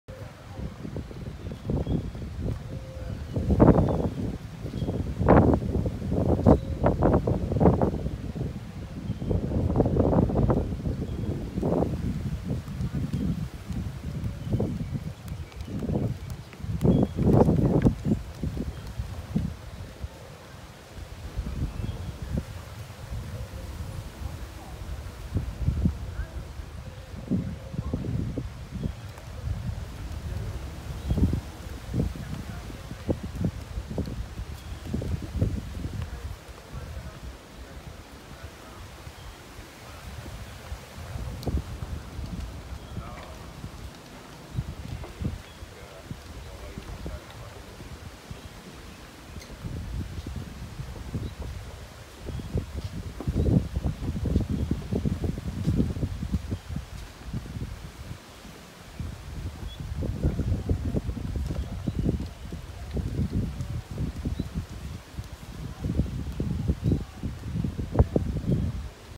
Dreams Riviera Cancun Resort, Mexico
Dreams Riviera Cancun Resort, Mexico on March 6, 2018.  Main walk area in the morning.